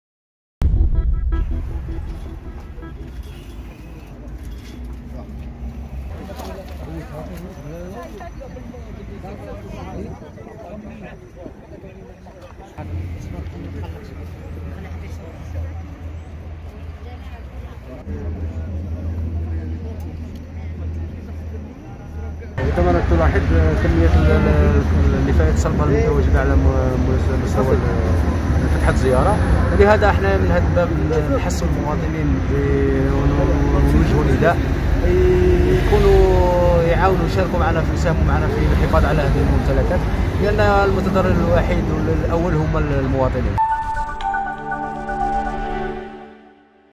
وهران : عملية تنظيف للبالوعات تحسبا لأي إضطرابات جوية
وهران (وأج) - أطلقت شركة المياه والتطهير لولاية وهران ''سيور'' عملية تنظيف واسعة تحسبا لأي إضطرابات جوية في الأيام القادمة.